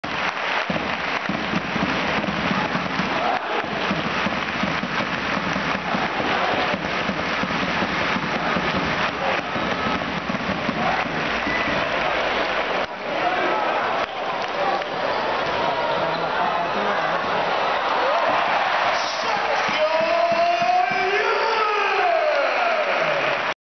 Siempre Sergio Llull
Ultimos segundos del partido, pero esta vez el segundo capitan madridsta, prefirio anotar debajo de la canasta que desde el triple,